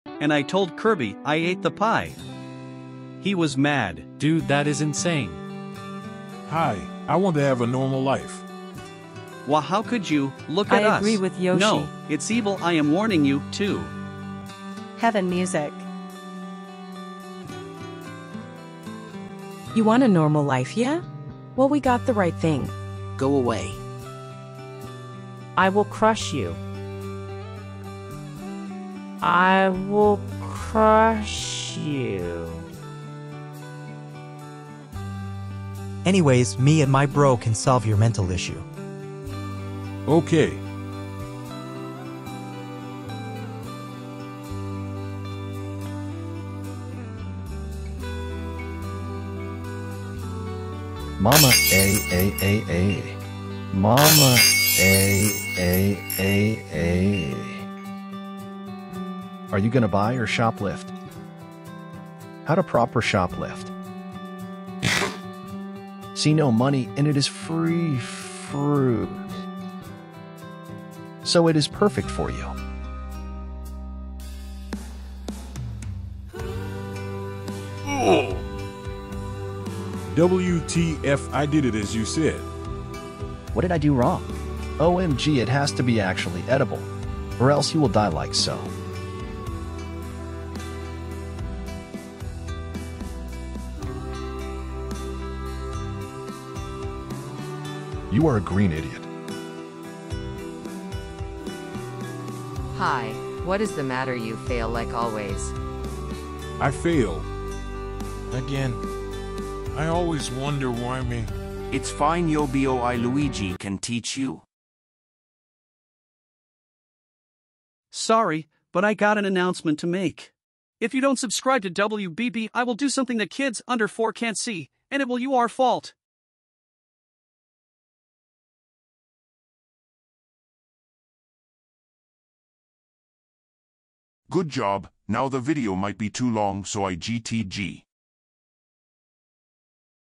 WBB Animations: TPGK&Y- Yoshi gets taught how to "shop lift" ft. Mario.
Anyways Enjoy the video, and more goofy ahh animations soon to come!